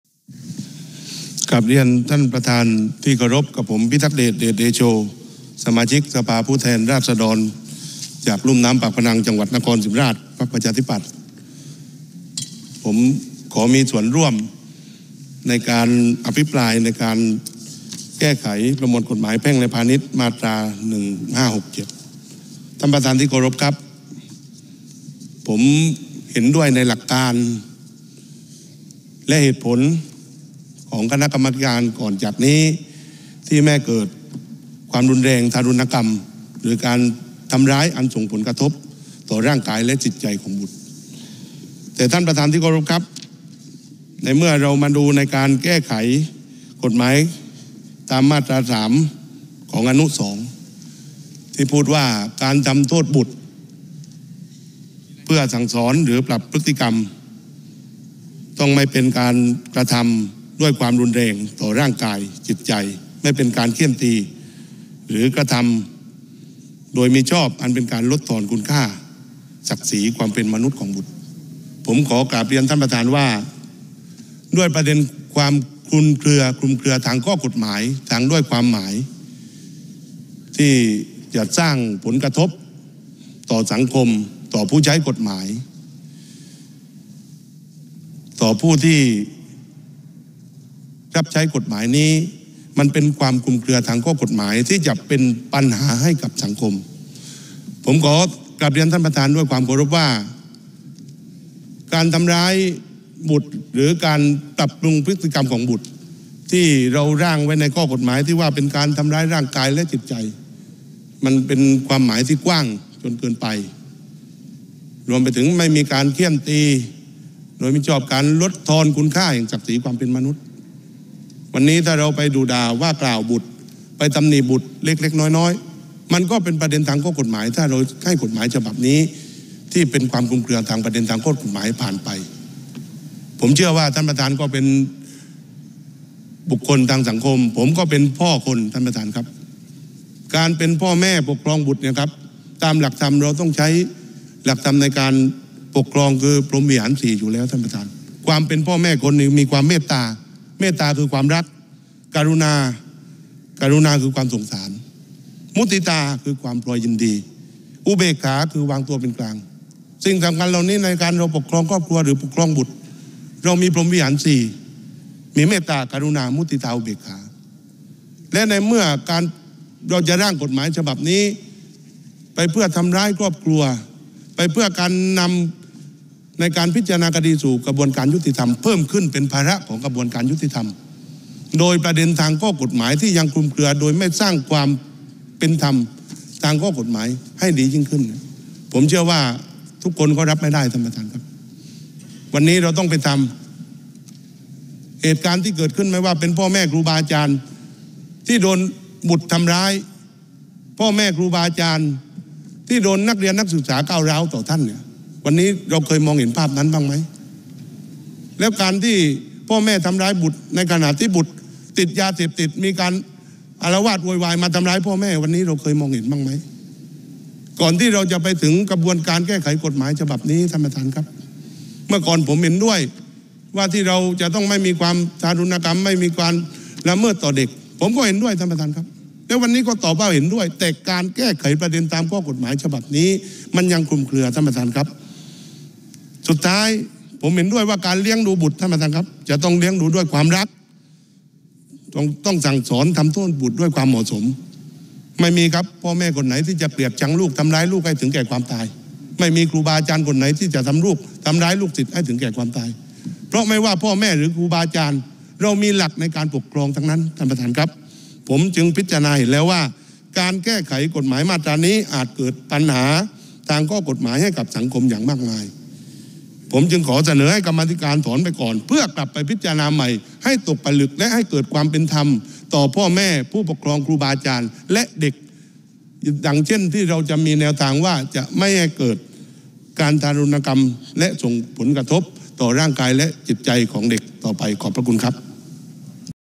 “พิทักษ์เดช” วอน กมธ. พิจารณา กม.ห้ามผู้ปกครองตีลูกใหม่ ให้ตกผลึกและให้เกิดความเป็นธรรม
(25 ก.ย. 67) นายพิทักษ์เดช เดชเดโช สส.นครศรีธรรมราช พรรคประชาธิปัตย์ อภิปรายในการประชุมสภาผู้แทนราษฎรเพื่อพิจารณาร่างพระราชบัญญัติแก้ไขเพิ่มเติมประมวลกฎหมายแพ่งและพาณิชย์ (ฉบับที่ ..) พ.ศ. มาตรา 3 โดยระบุว่า
เห็นด้วยในหลักการและเหตุผลของคณะกรรมาธิการ ที่ไม่ให้เกิดความรุนแรงทารุณกรรมหรือการทำร้ายอันส่งผลกระทบต่อร่างกายและจิตใจของบุตร ในเมื่อมาดูในการแก้ไขกฎหมายตามมาตรา 3 (2) ที่พูดว่า “...การทำโทษบุตรเพื่อสั่งสอนหรือปรับพฤติกรรม โดยต้องไม่เป็นการกระทำด้วยความรุนแรงต่อร่างกาย จิตใจ ไม่เป็นการเฆี่ยนตี หรือกระทำโดยมิชอบ อันเป็นการรบกวนคุณค่าศักดิ์ศรีความเป็นมนุษย์ของบุตร...”
ด้วยประเด็นความคลุมเครือทางข้อกฎหมายและคลุมเครือด้วยความหมาย ที่จะสร้างผลกระทบต่อสังคมต่อผู้ใช้กฎหมายต่อผู้ที่บังคับใช้กฎหมายนี้ มันเป็นความคลุมเครือทางข้อกฎหมายที่จะเป็นปัญหาให้กับสังคม
“การทำร้ายบุตรหรือการปรับปรุงพฤติกรรมของบุตร” ที่ร่างไว้ในข้อกฎหมายที่ว่าเป็นการทำร้ายร่างกายและจิตใจ มันเป็นความหมายที่กว้างจนเกินไป รวมถึง “ไม่มีการเฆี่ยนตีโดยไม่ชอบลดทอนคุณค่าแห่งศักดิ์ศรีความเป็นมนุษย์” วันนี้ถ้าเราไปดุด่าบุตร ไปตำหนิบุตรเล็ก ๆ น้อย ๆ  มันก็เป็นประเด็นทางข้อกฎหมาย ถ้าเราให้กฎหมายฉบับนี้ที่เป็นประเด็นความคลุมเครืทางข้อกฎหมายผ่านไป
นายพิทักษ์เดช กล่าวต่ออีกว่า ส่วนตัวตนก็เป็นพ่อคน การเป็นพ่อแม่ปกครองบุตรตามหลักธรรม เราต้องใช้หลักธรรมในการปกครอง คือ พรหมวิหาร 4 ความเป็นพ่อแม่คน มี “เมตตา” คือ ความรัก “กรุณา” คือ ความสงสาร “มุทิตา” คือ ความพลอยยินดี และ “อุเบกขา” คือ การวางตัวเป็นกลาง สิ่งสำคัญเหล่านี้ ในการปกครองครอบครัวหรือการปกครองบุตร เราต้องมีพรหมวิหาร 4 และในเมื่อจะร่างกฎหมายฉบับนี้ไปเพื่อทำร้ายครอบครัว ไปเพื่อการนำในการพิจารณาคดีสู่กระบวนการยุติธรรมเพิ่มขึ้น เป็นภาระของกระบวนการยุติธรรม โดยประเด็นทางข้อกฎหมายที่ยังคลุมเครือ โดยไม่สร้างความเป็นธรรมทางข้อกฎหมายให้ดียิ่งขึ้น เชื่อว่าทุกคนก็รับไม่ได้
วันนี้เราต้องไปทำเหตุการณ์ที่เกิดขึ้น ไม่ว่าจะเป็นพ่อแม่ครูบาอาจารย์ที่โดนบุตรทำร้าย พ่อแม่ครูบาอาจารย์ที่โดนนักเรียนนักศึกษาก้าวร้าว วันนี้เราเคยมองเห็นภาพนั้นบ้างหรือไม่ แล้วการที่พ่อแม่ทำร้ายบุตร ในขณะที่บุตรติดยาเสพติดหรือมีการอาละวาดโวยวาย มาทำร้ายพ่อแม่ วันนี้เราเห็นภาพนี้กันบ้างไหม ก่อนที่เราจะไปถึงกระบวนการแก้ไขกฎหมายฉบับนี้ เมื่อก่อนส่วนตัวเห็นด้วยว่า ที่เราจะต้องไม่มีการทารุณกรรม ไม่มีการละเมิดต่อเด็ก ส่วนตัวเห็นด้วย แล้ววันนี้ก็ยังตอบว่าเห็นด้วย แต่การแก้ไขประเด็นทางข้อกฎหมายฉบับนี้มันยังมีความคลุมเครือ
นายพิทักษ์เดช เห็นด้วยว่า การเลี้ยงดูบุตร จะต้องเลี้ยงดูด้วยความรัก ต้องสั่งสอนทำโทษบุตรด้วยความเหมาะสม เพราะไม่มีพ่อแม่คนไหน ที่จะเกลียดทั้งลูกหรือทำร้ายลูกจนถึงแก่ความตาย ไม่มีครูบาอาจารย์คนไหนที่จะทำร้ายลูกศิษย์ จนถึงแก่ความตาย เพราะไม่ว่าพ่อแม่หรือครูบาอาจารย์ เรามีหลักในการปกครองทั้งนั้น
จึงพิจารณาเห็นแล้วว่า การแก้ไขกฎหมายมาตรานี้ อาจเกิดปัญหาทางข้อกฎหมายให้กับสังคมอย่างมากมาย จึงขอเสนอให้คณะกรรมาธิการถอนออกไปก่อน เพื่อพิจารณาใหม่ให้ตกผลึกและให้เกิดความเป็นธรรม ต่อพ่อแม่ผู้ปกครองครูบาอาจารย์และเด็ก อย่างเช่นที่เราจะมีแนวทางว่า ไม่ให้เกิดการทารุณกรรมและส่งผลกระทบต่อร่างกายและจิตใจของเด็กต่อไป